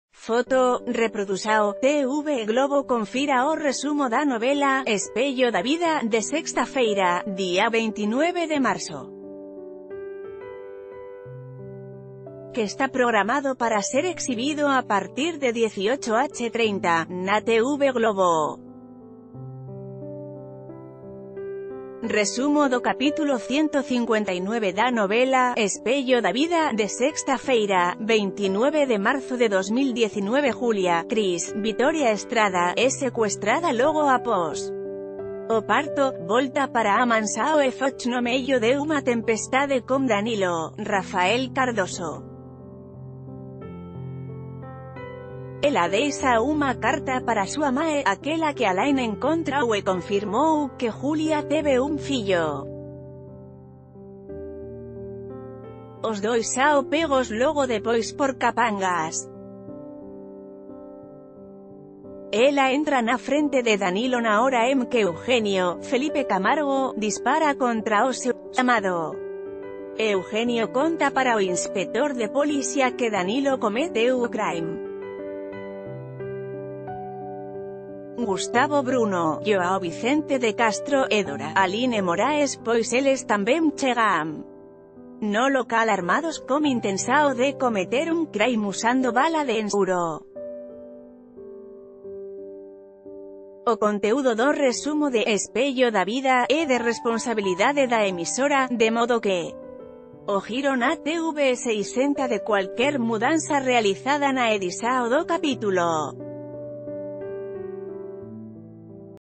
Resumo da novela “Espelho da Vida” – sexta-feira, dia 29/03
Foto: Reprodução/TV Globo Confira o resumo da novela “Espelho da Vida” de sexta-feira, dia 29 de Março, que está programado para ser exibido a partir de 18h30, na TV Globo. Resumo do capítulo 159 da n...